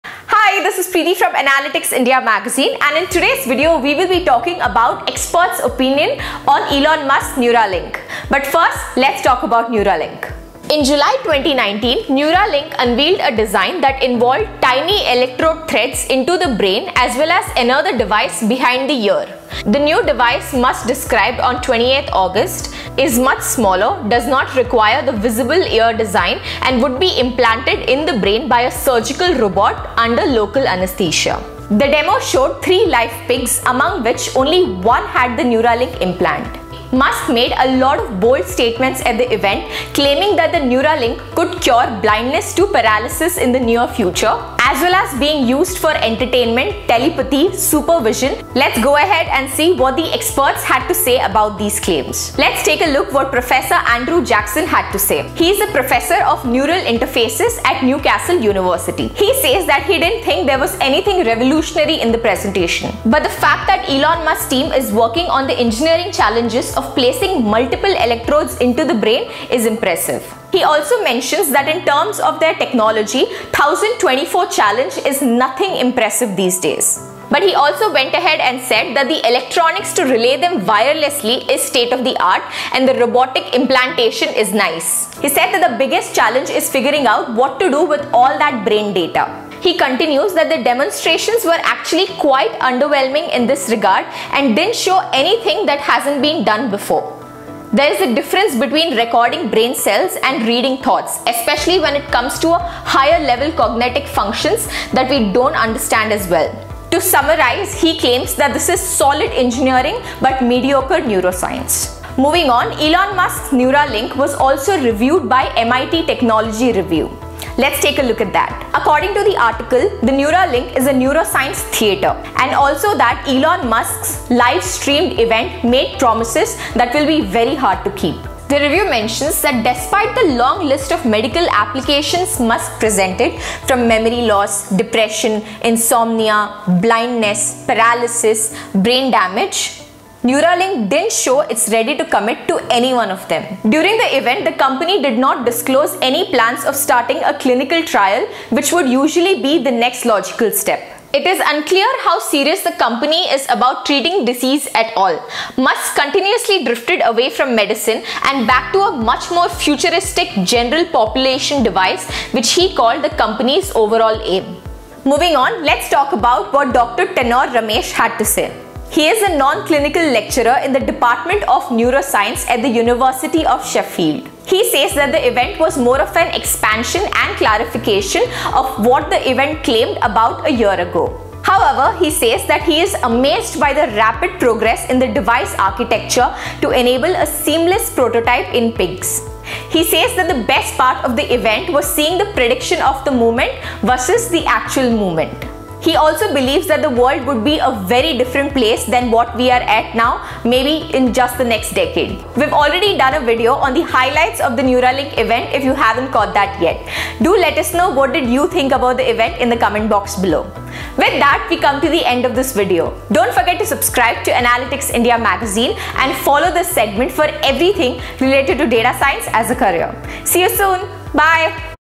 Elon Musk's- Neuralink - A HIT or a MISS
Elon Musk has time and again proven to be the real-life Ironman. The success of SpaceX to the ambitious projects by Boring company. Elon Musk has always surprised us with some of the greatest innovations.

This time  Elon Musk gave a live demo on August 28th on Neuralink.

The demo involved three live pigs, only one of which actually had a Neuralink device in its brain. Musk made a lot of bold claims during the event, like Neuralink someday treating blindness and paralysis, as well as being used for entertainment, supervision or even memory retrieval etc. Let’s see what experts in the industry had to say about these claims.

10 Mind-Bending Applications Of Neuralink, According To The Team

10 Interesting Things Elon Musk Said At The Neuralink Event

How Neuralink’s Human Trials Can Be Beneficial For Humanity